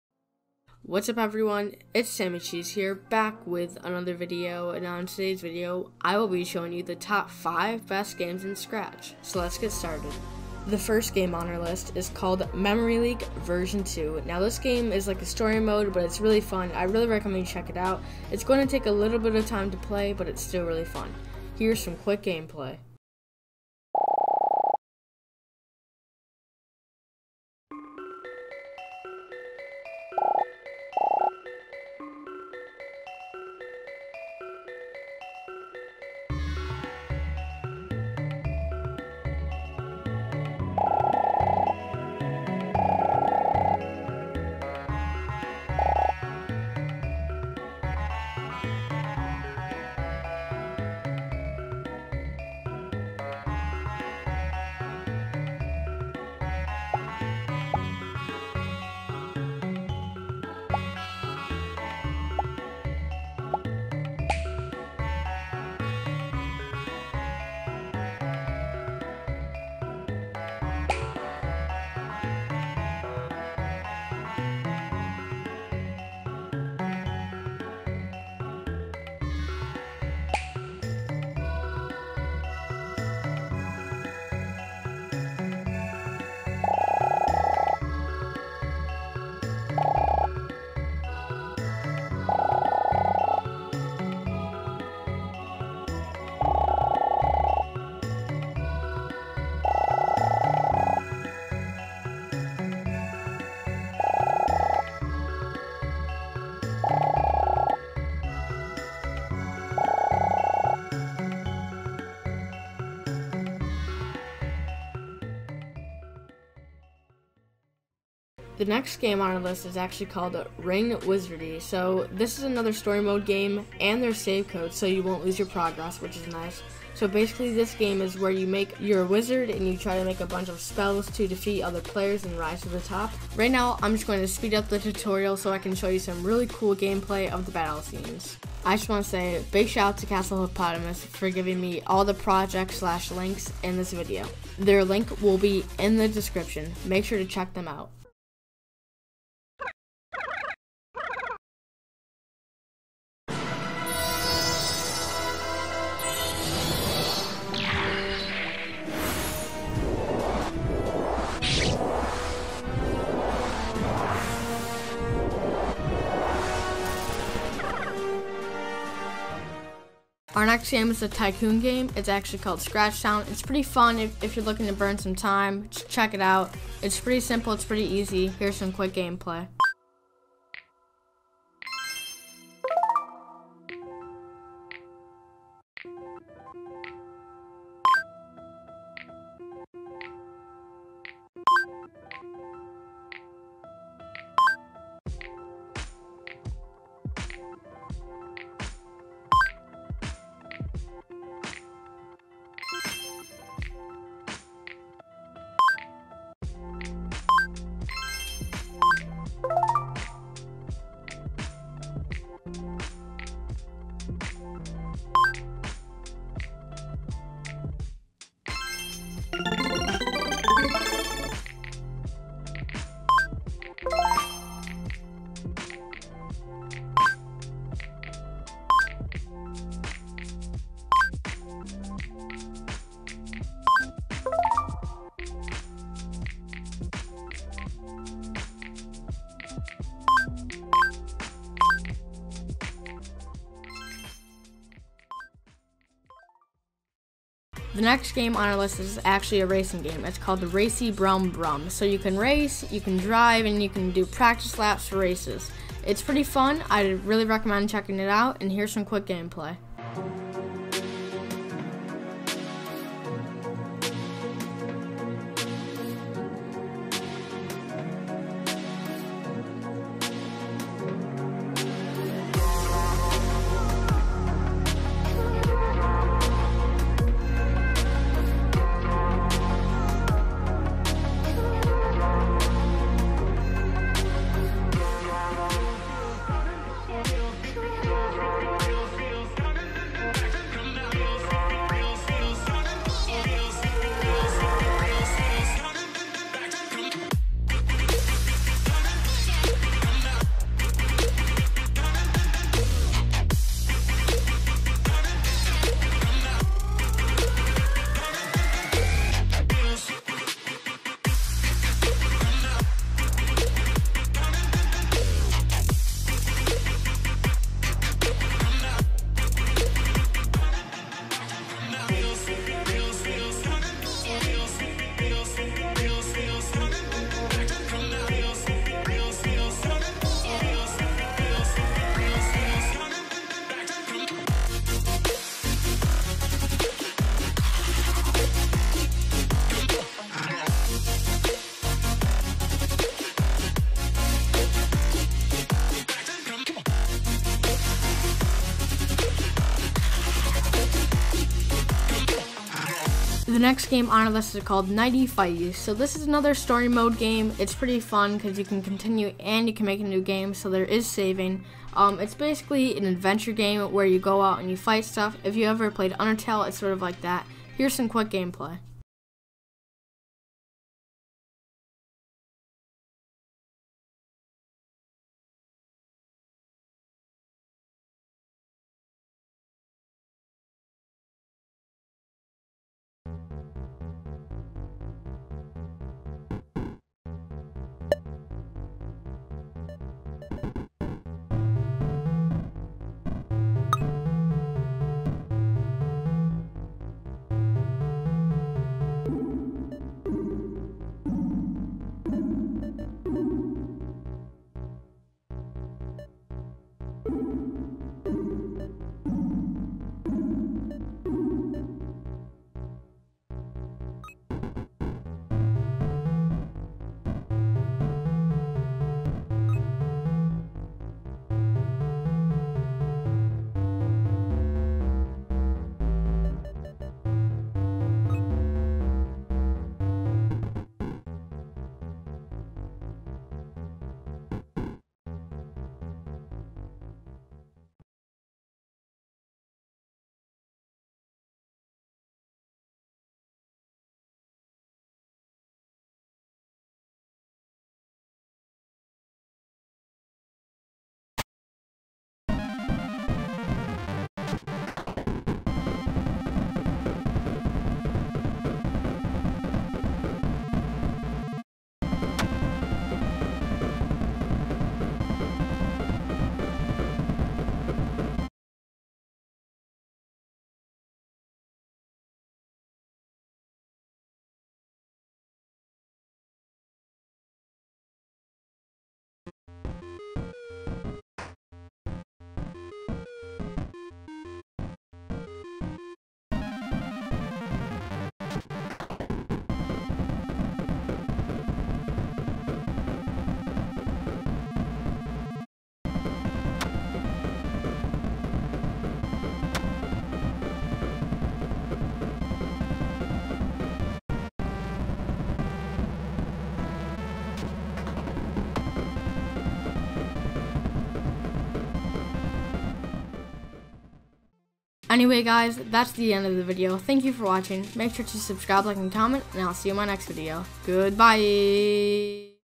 Top 5 BEST Games In Scratch!
What's up everyone, this is SammyCheez here, back with another video. In today's video, I will be showing you the top 5 best/coolest games in Scratch! Most of the games in this video were suggested by @Castle_Hippopotamus. Please note that I did not create/make/help with any of these games in the video.

🎮GAMES MENTIONED IN THE VIDEO🎮

Thanks to @RokCoder for the thumbnail!

Scratch is developed by the Lifelong Kindergarten Group at the MIT Media Lab.